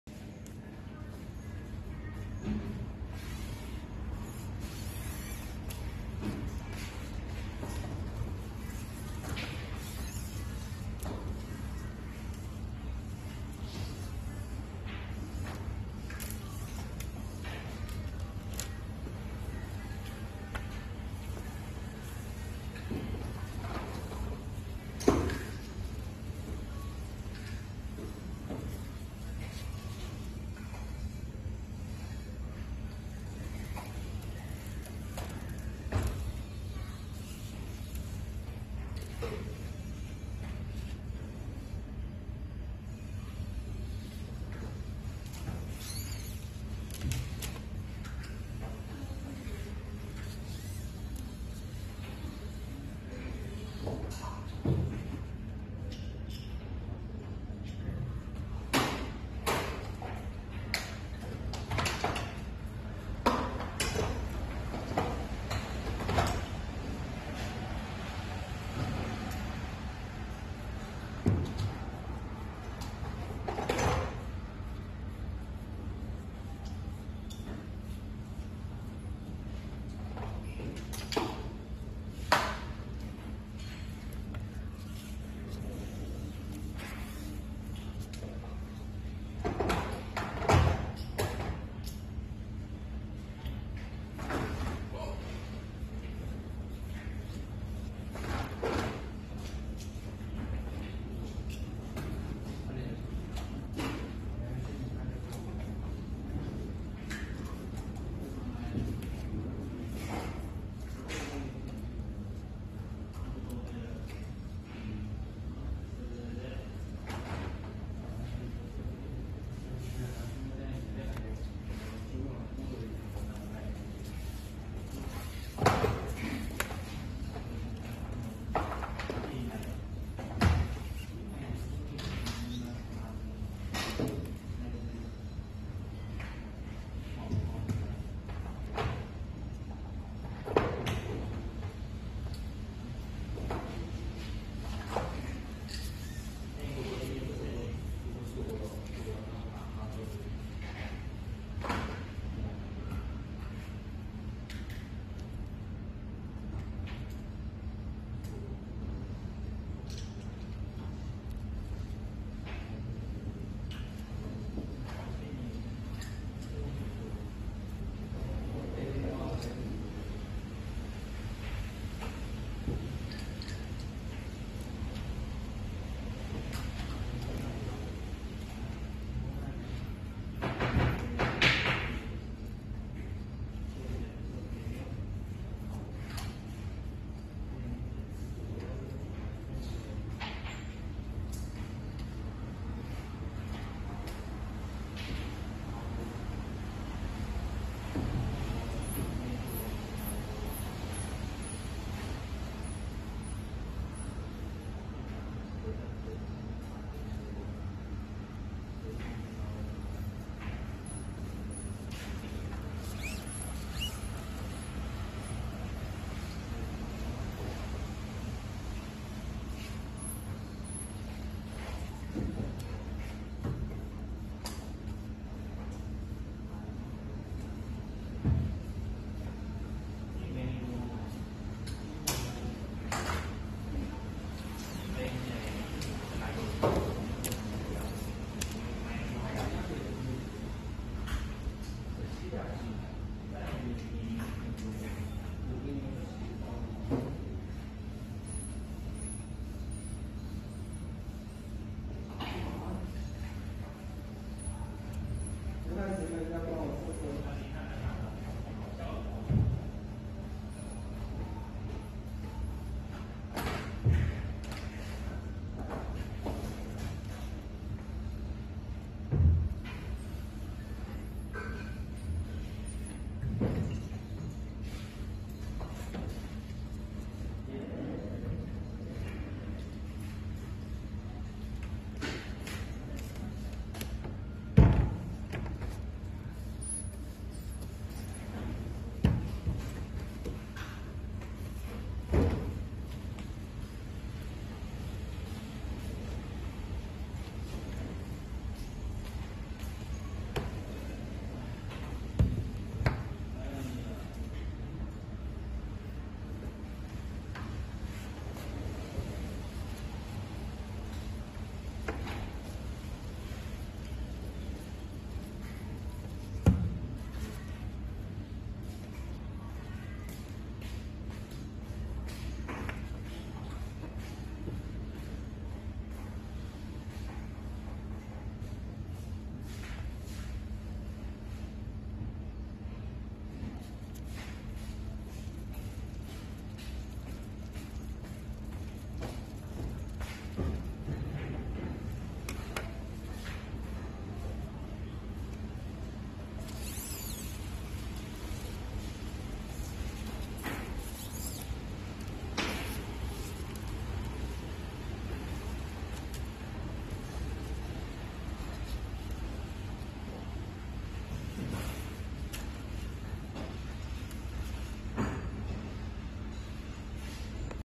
FY-MED Combine the Coil Pipe Assembly
The above video can teach you how to weld Coil Pipe and bending section.
We focus on various endoscope-related equipment repairs.